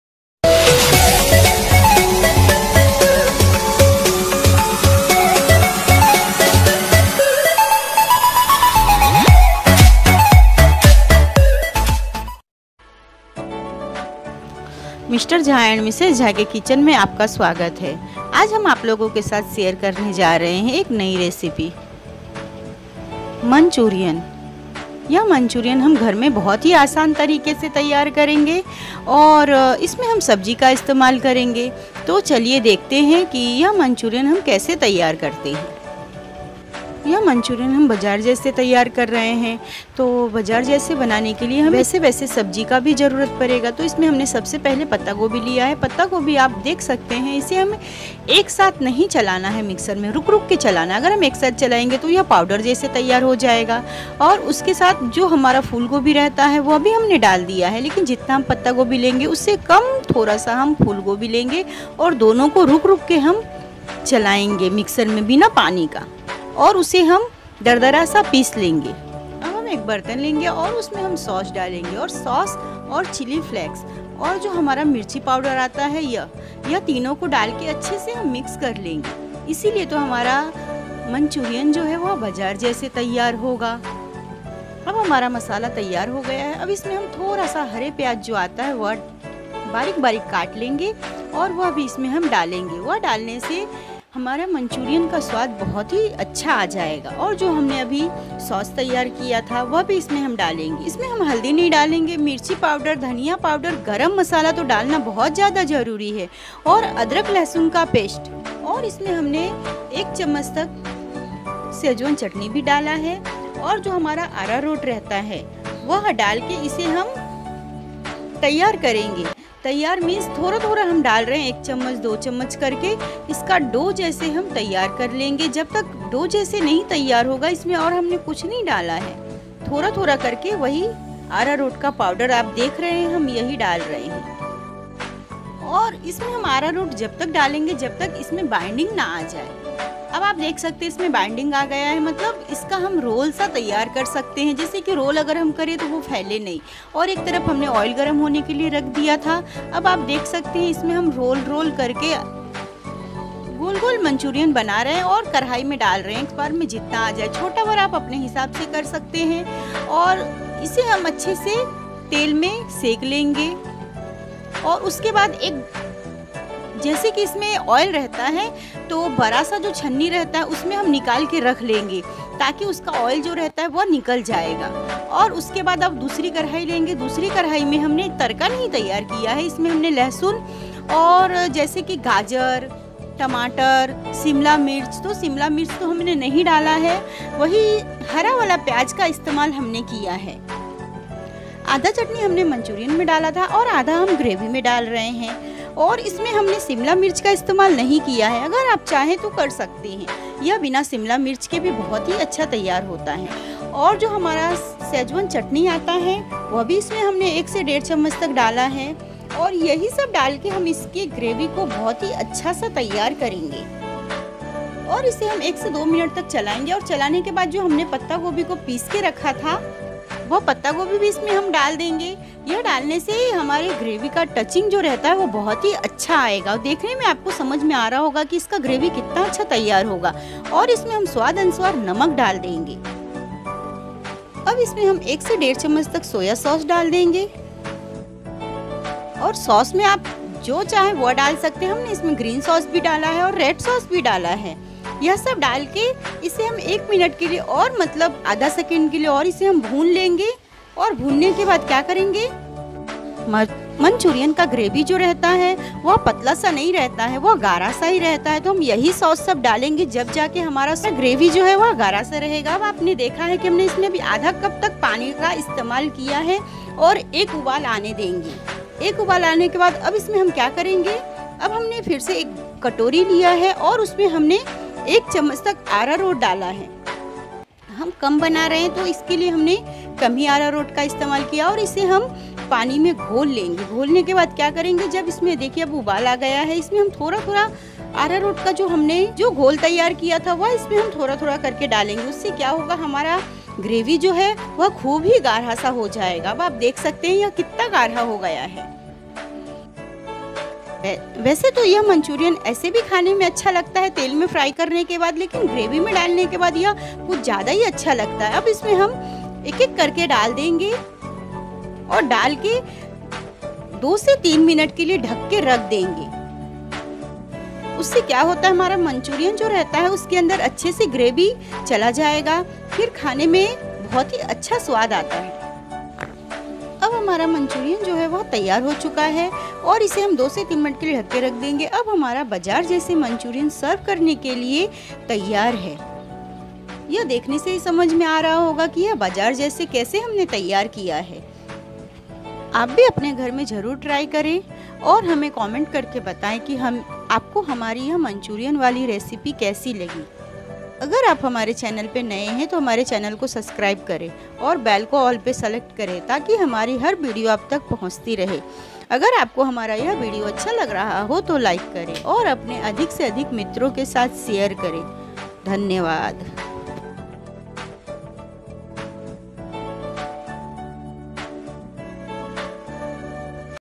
Veg manchurian ki recipe |How to make veg manchurian at home | घर मे आसानी से बनाएँ मंचूरियन
Hello dosto❤ Mera naam hai kk jha(journalist) Mai apne is cooking channel Mr. jha and Mrs. jha pe kuch unique recipes post karta hu. To aaj hamne banaya hai वेग मंचूरियन .

Facebook ID : Krishna kumar jha Pura

DISCRIPTION padhne ke liye dhanyawad❤🙏🏻